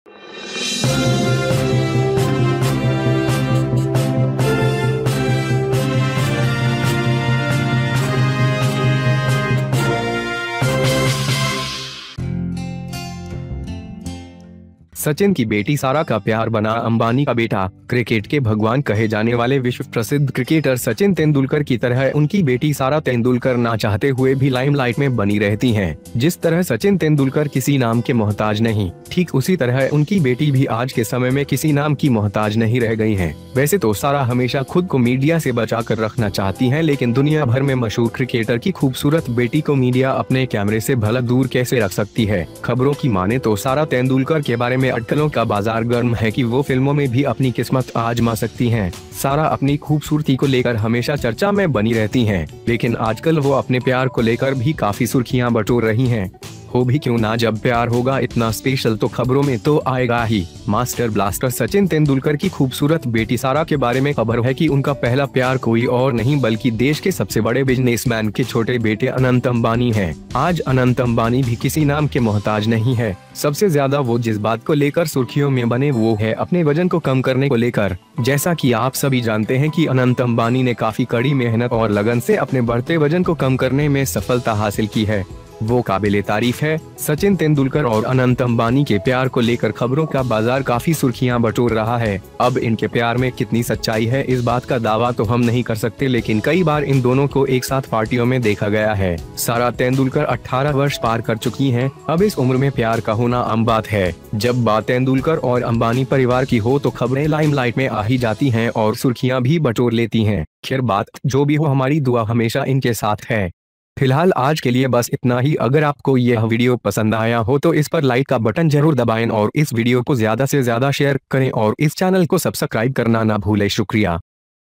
अंबानी के तीसरे बेटे अनंत अंबानी को इस लड़की पर हुआ प्यार.
Like the world famous cricketer Sachin Tendulkar, the son of Sachin's daughter Sara, the son of cricket, like Sachin Tendulkar, does not want his daughter, Sarla Tendulkar, remains in Limelite, just like Sachin Tendulkar is not like any name, just like that Like his daughter too, there is no love for any name in today's time, though, Sarah always wants to protect herself from the media but in the world How can the media get rid of the beautiful daughter of the Hurricane cricketer, as reports say that the market of speculation about all Tendulkar is hot that they can also try their luck in movies, Sarah always discusses her beauty It remains, but nowadays, they are getting a lot of headlines about their love.

AK News